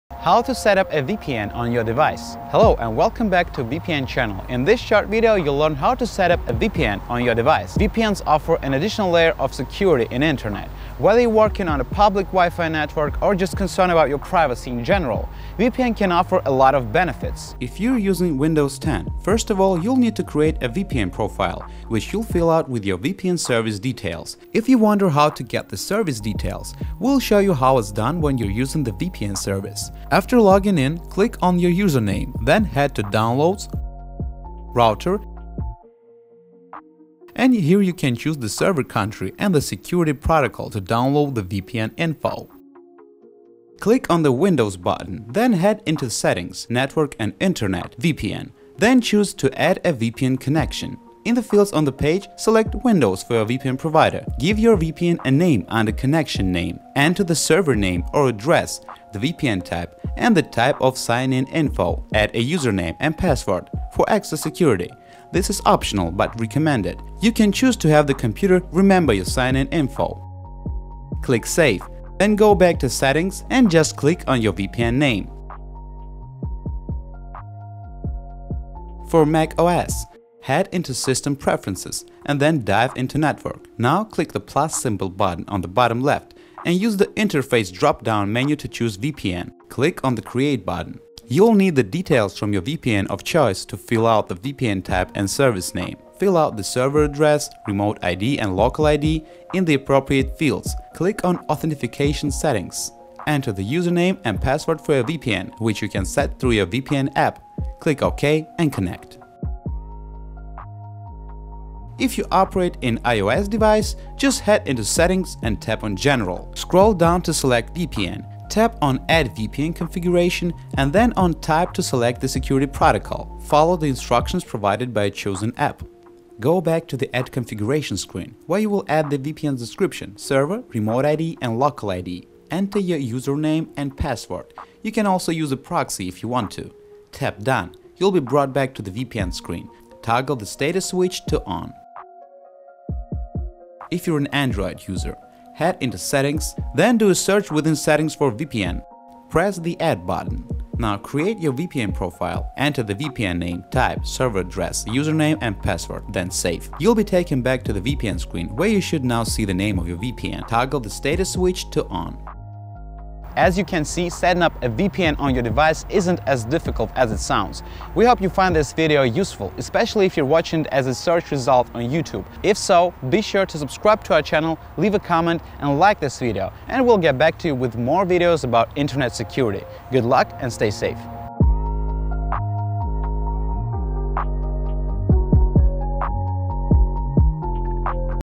How to setup a VPN on your device
In this short tutorial video you’ll learn how to set up VPN on your device (PC/Windows 10, iOS, Android, MacOS).

With a fast VPN you can access all Internet resources on any of your devices. VeePN will be your best friend in the Internet space of any country, media stream, game and any content.

0:00 - How to setup a VPN on your device
0:26 - How to get your VPN service details
0:44 - How to setup a VPN on Windows 10
1:22 - How to setup a VPN  on MacOS
1:52 - How to setup a VPN on  iOS
2:25 - How to setup a VPN  on Android